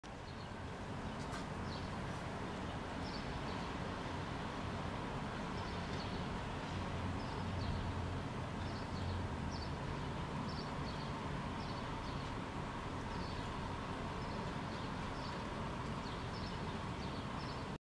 Wood Pigeon drinking from the puddle; taking off
Common Wood Pigeon (Columba palumbus) drinking from the puddle; taking off.

Sepelkyyhky (Columba palumbus) juo vettä lätäköstä; lähtee lentoon.